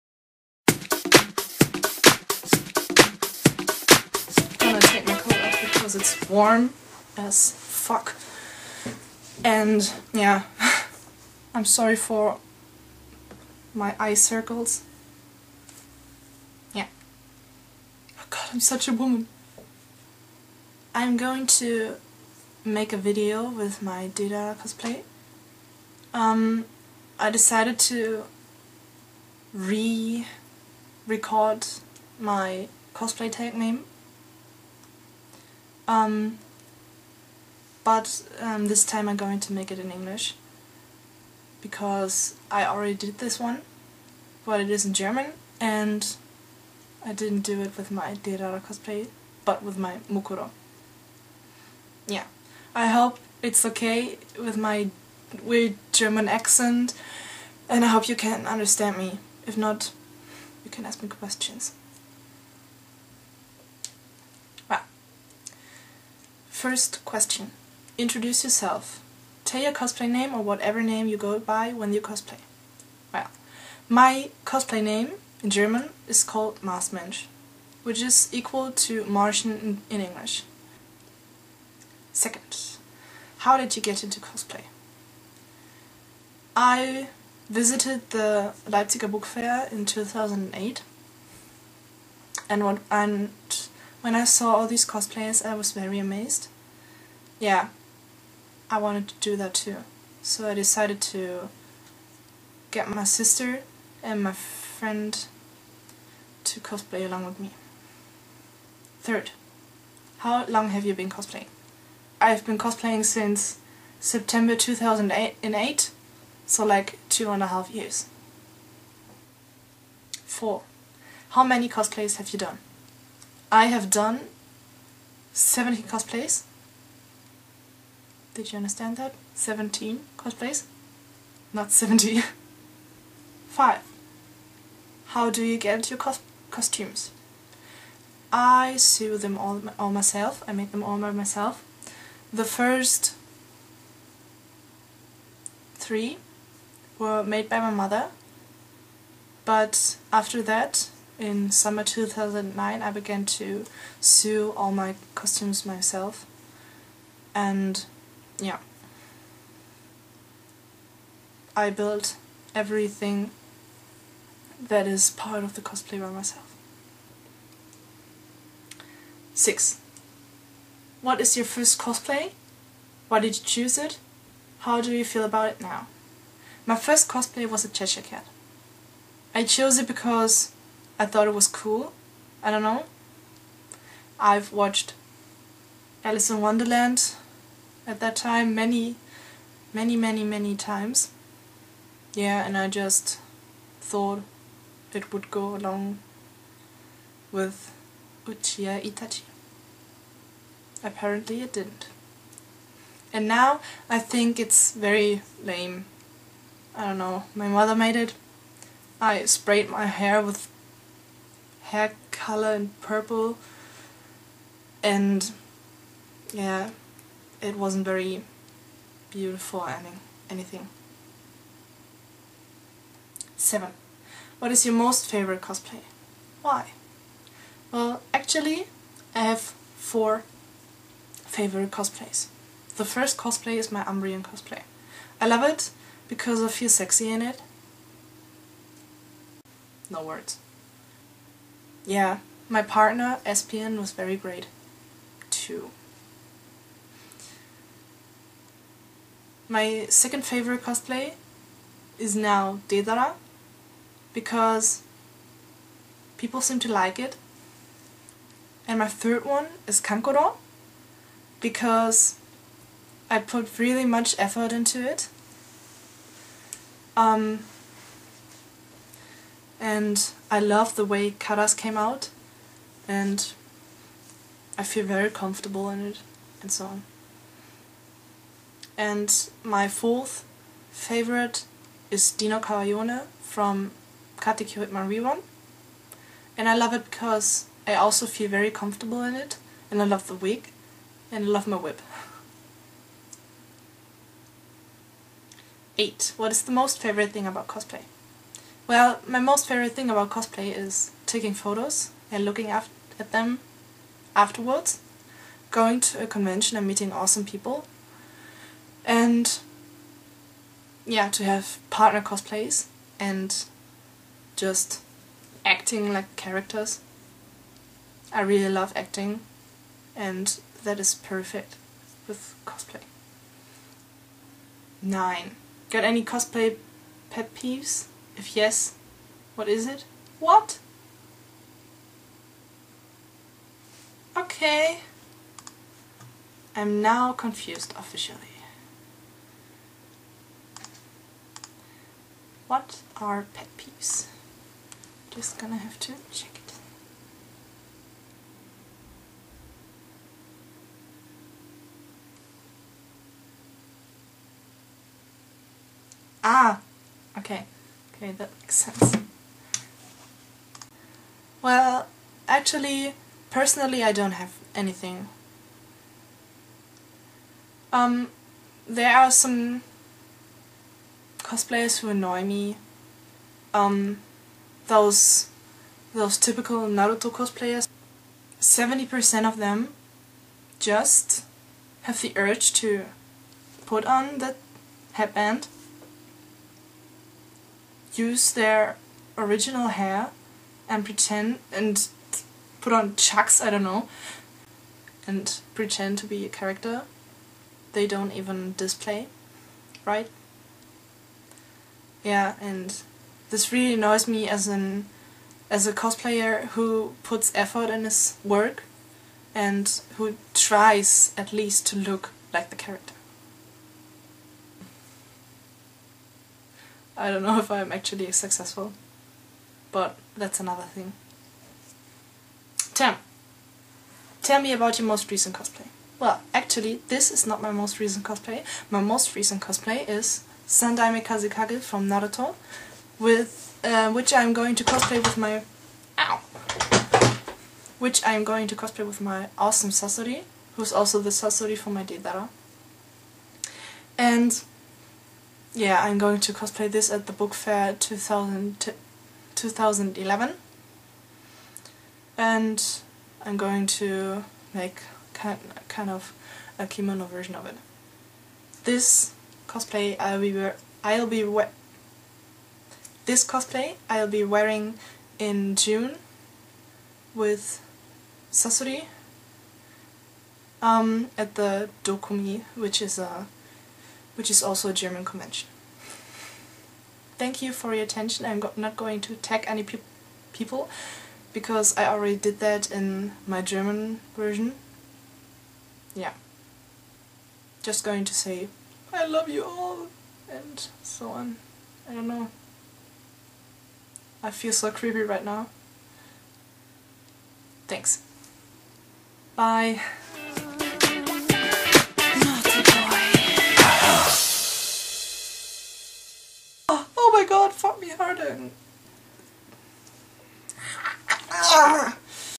Cosplay Tag
Well, I wanted to make a video with my Deidara Cosplay and since I hadn't got any ideas, I decided to remake my Cosplay Meme in English.

I'm sorry for my terrible english and my stammering, but I felt very uncomfortable and I couldn't really talk properly.

I was also very nervous, which showed in touching my face and moving around .. haha, I might be hyperactive ..

Also, too little Sasori ( the cosplayer ) appreciation! I'm sorry! ;_;

I'm terribly sorry for not being funny and entertaining at all!